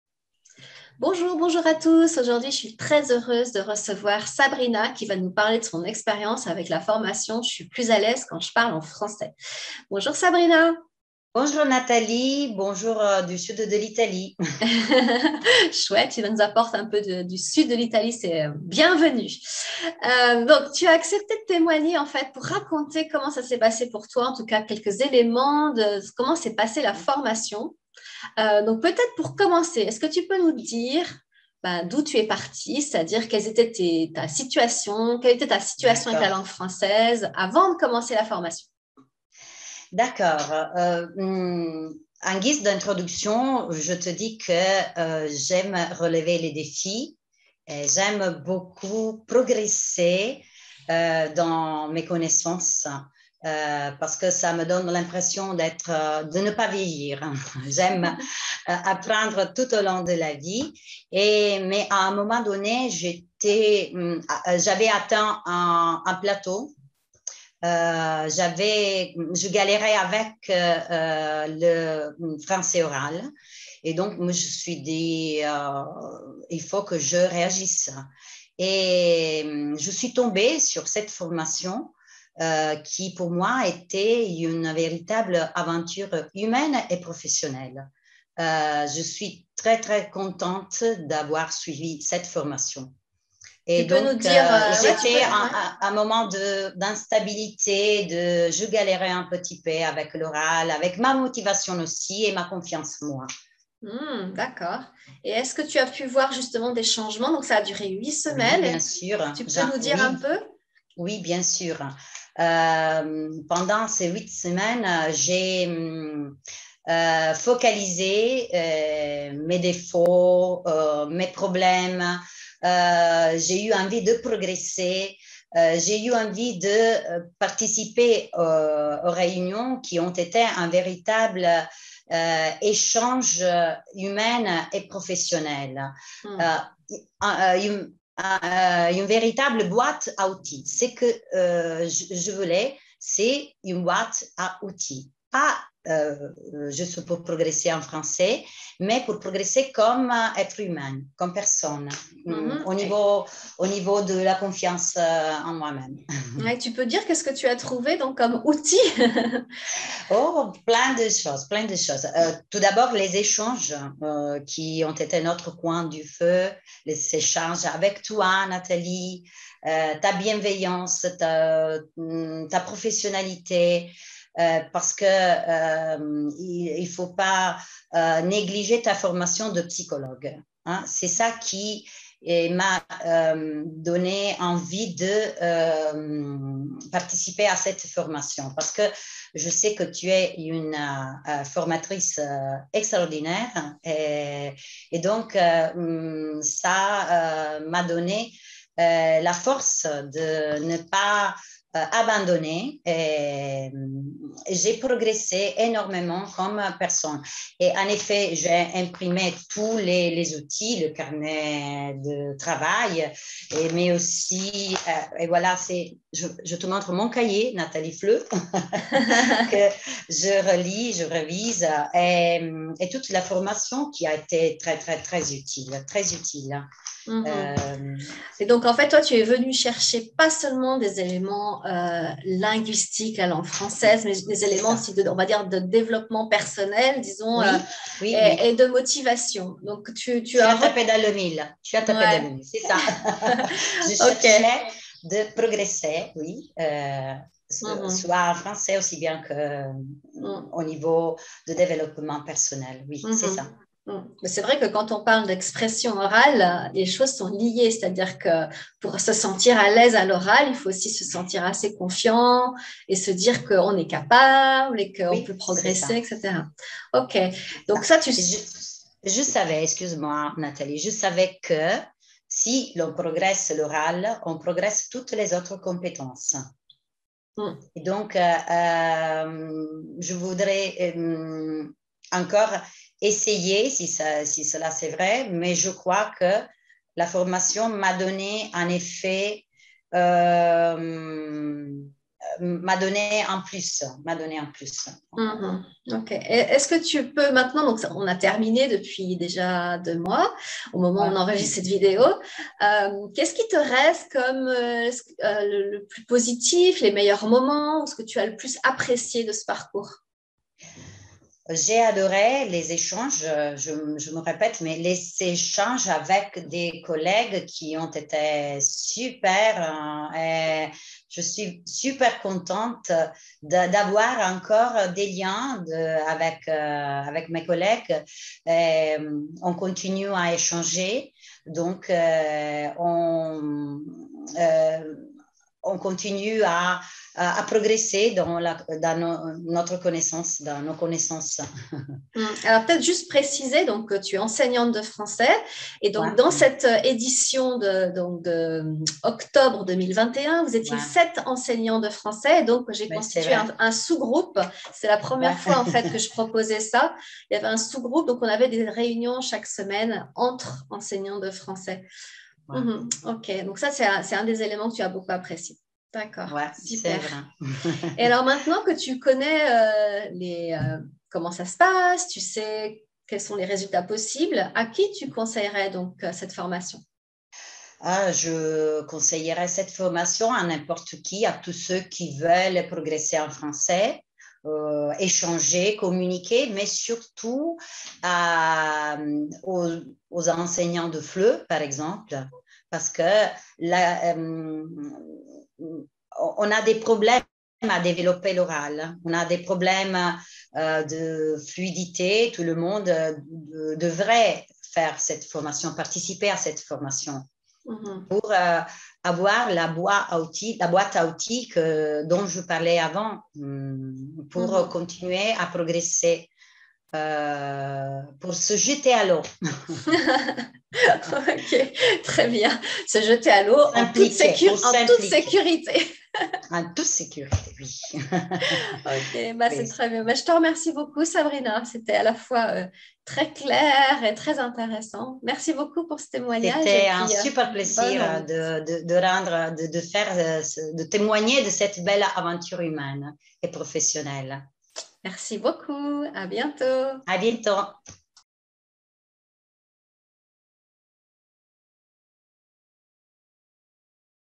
Témoignage Sabrina Spagnolo : "une véritable aventure humaine et professionnelle"
Sabrina est enseignante de FLE et voulait continuer de progresser en français et avoir plus confiance en elle à l'oral.
Elle raconte ce que lui a apporté le programme Destination Confiance "Je suis plus à l'aise à l'oral" : "une véritable aventure humaine et professionnelle !"

Sabrina a suivi la formation "Destination Confiance à l'oral / je suis plus à l'aise quand je parle en français".
8 semaines de formation en ligne avec un coaching de groupe en direct chaque semaine.

Prochaine session de formation : octobre 2025

Le coaching individuel est possible toute l'année.

RECEVEZ DES CONTENUS GRATUITS : inscrivez-vous à mon infolettre (informations par email)